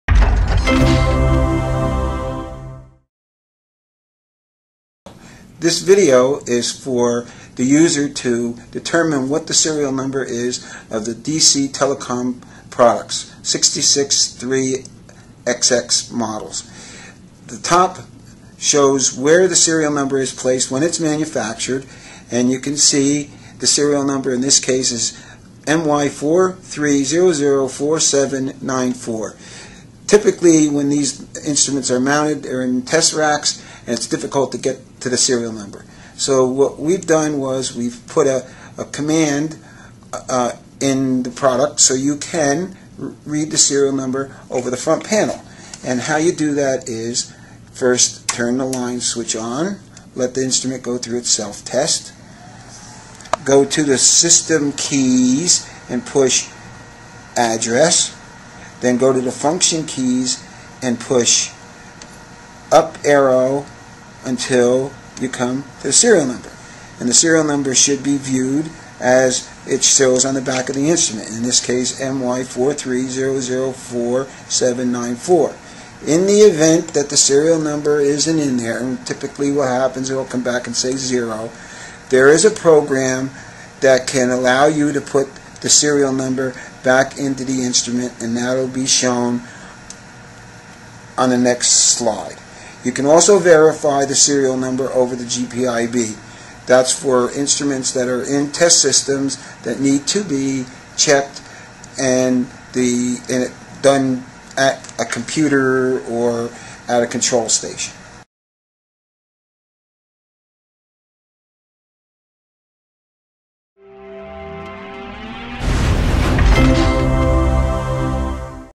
66300X Mobile communications Serial Number Verification
Agilent Moble Communication DC Power Supplies.
This video describes the procedure used to view the serial number on the front panel of the instrument.